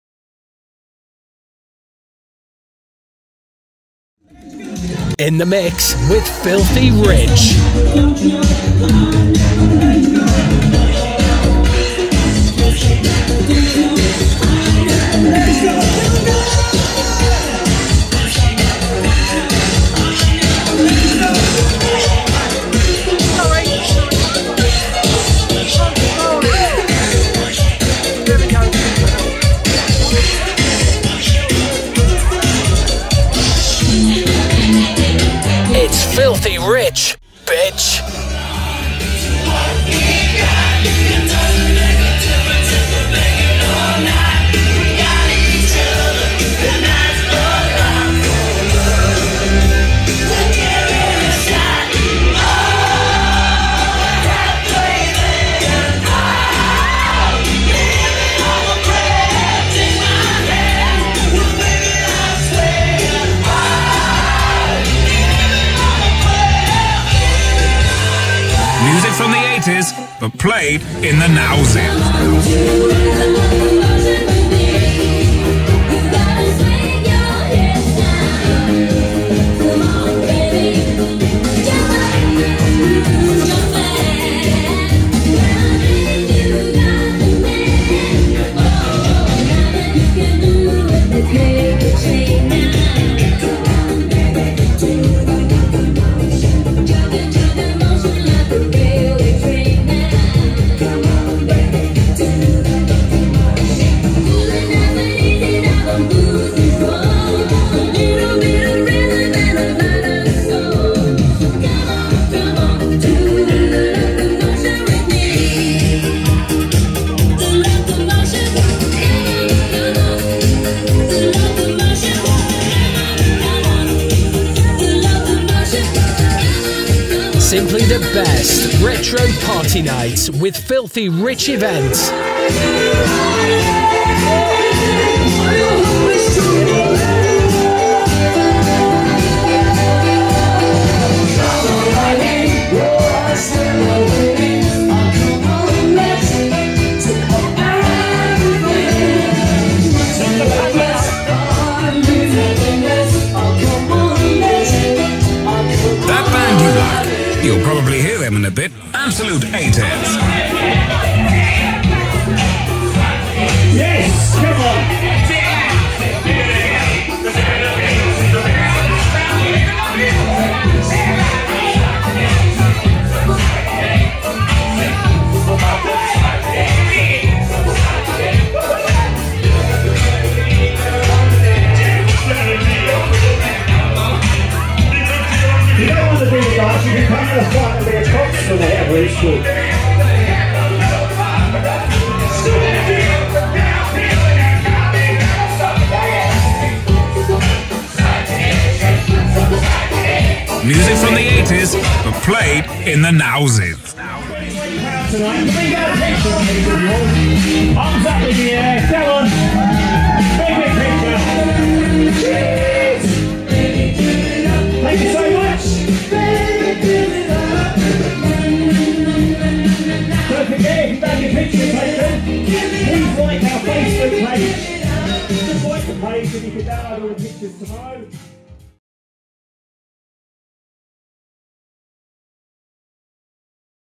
Ultimate 80s Party Night - May14th at Norwich Mercure hotel
Our first ever 80s Night in Norwich - and what a success it was!